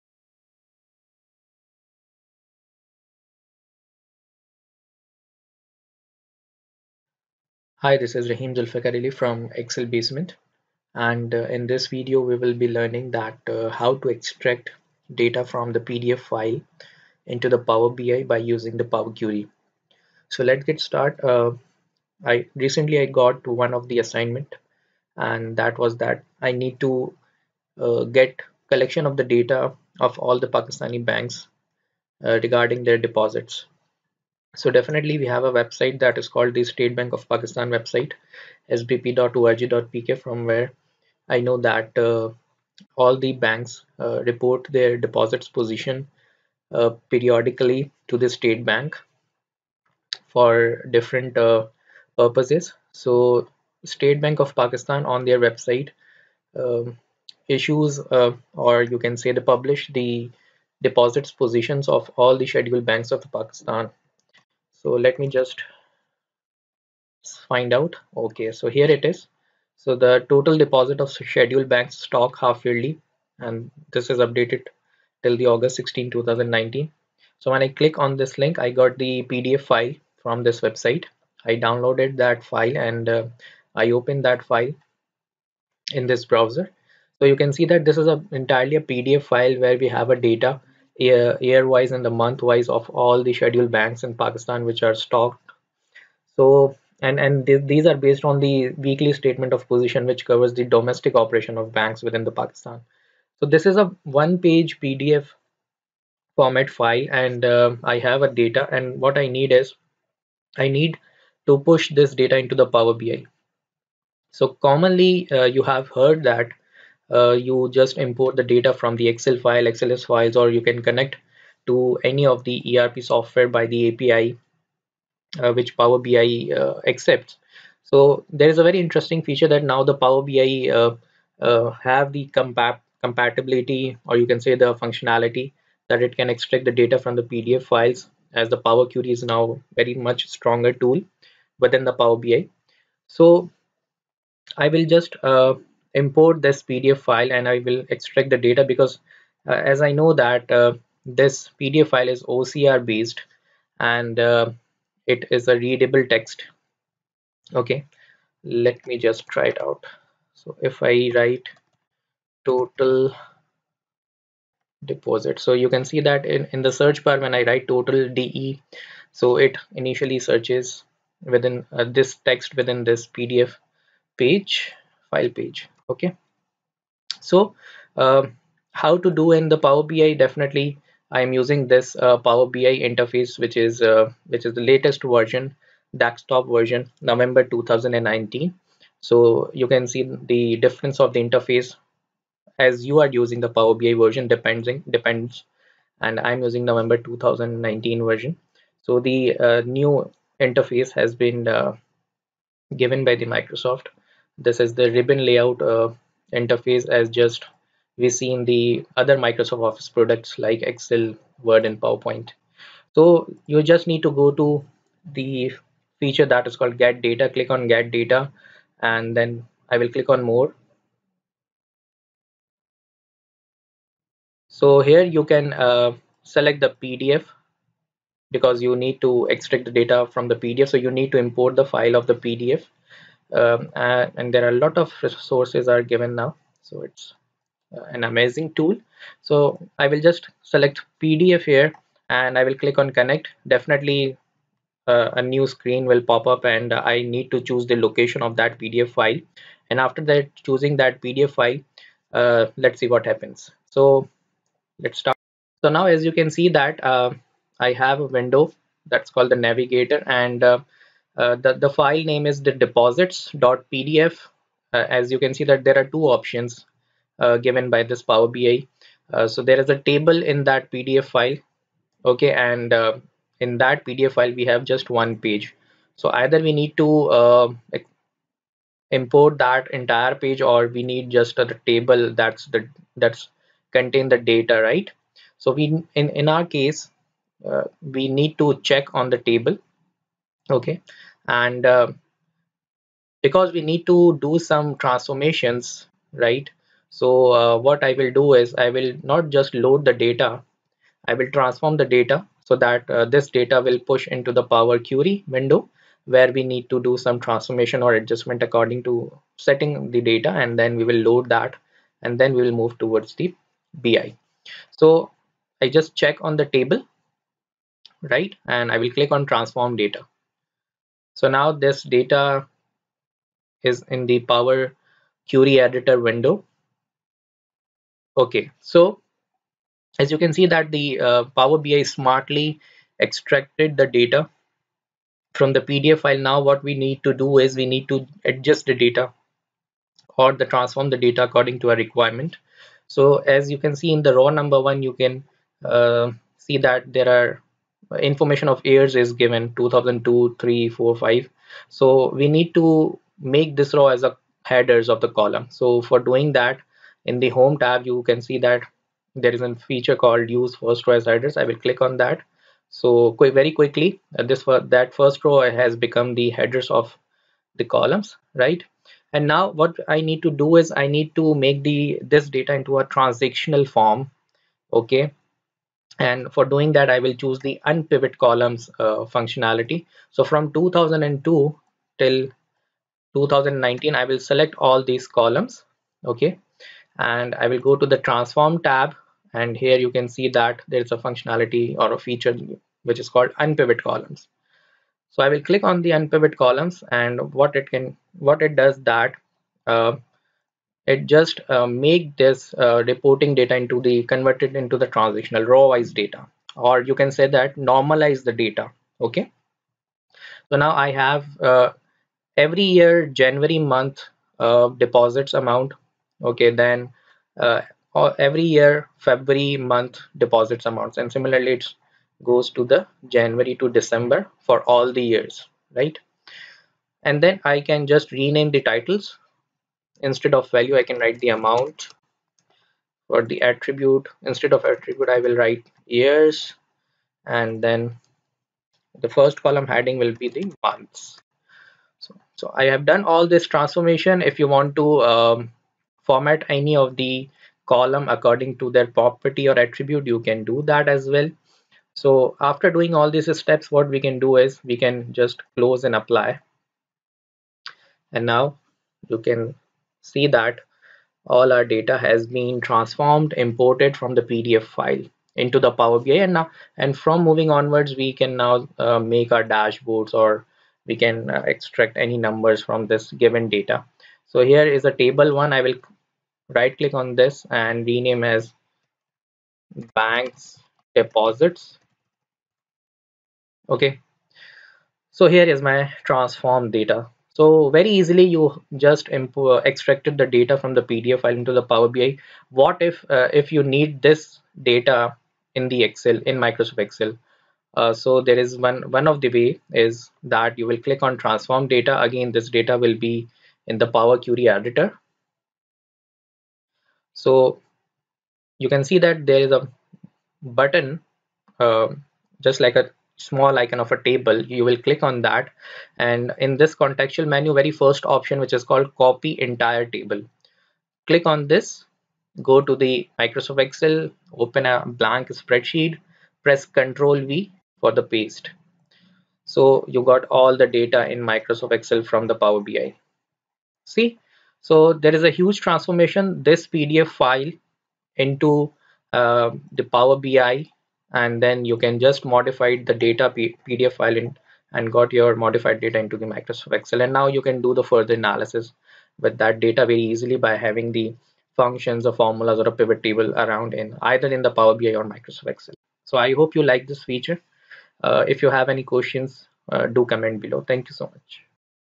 Extract Data from PDF Files with Power Query in Power BI & Also paste data in Excel
In this video, we're going to look at how we can extract data from PDF files with Power Query in Power BI.

We can then copy and paste the data into Excel.